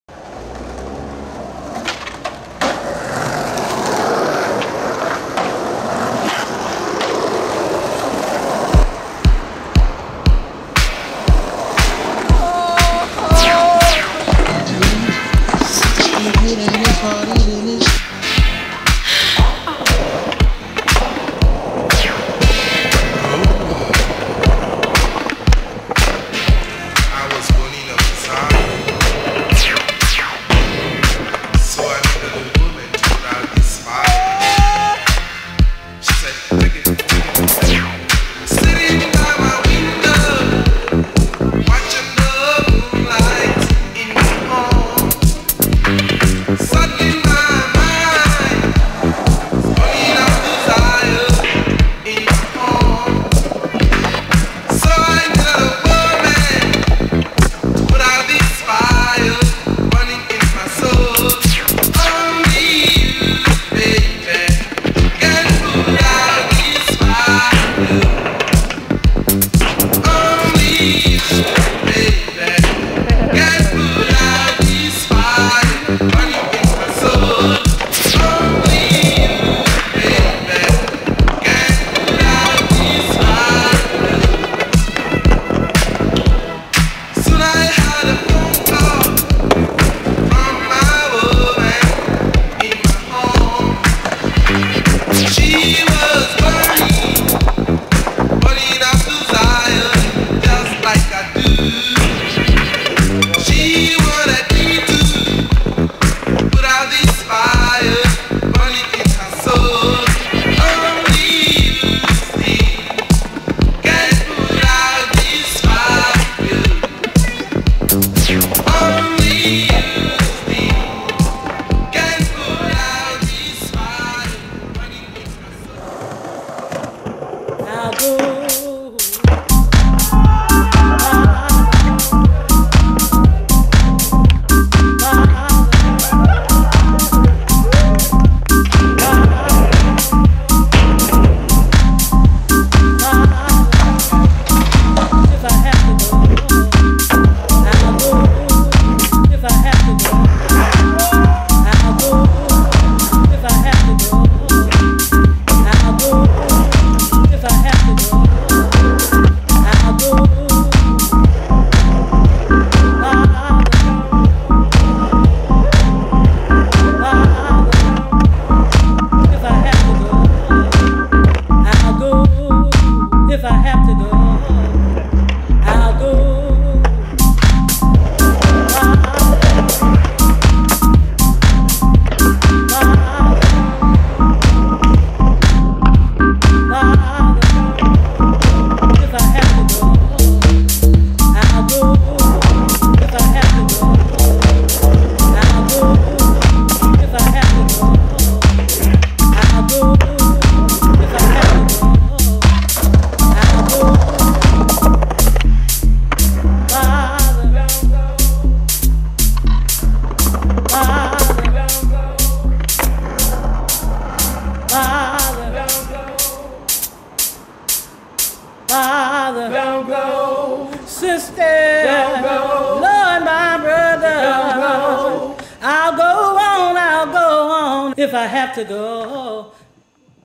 adidas – Summer Rave
The German adidas squad in Berlin feat. Daniel Ledermann, Patrick Rogalski, Jost Arens, Philipp Oehmige, Sascha Scharf and Ike Fromme.

Produced by: Torsten Frank

Filmed & Edited: Dennis Ludwig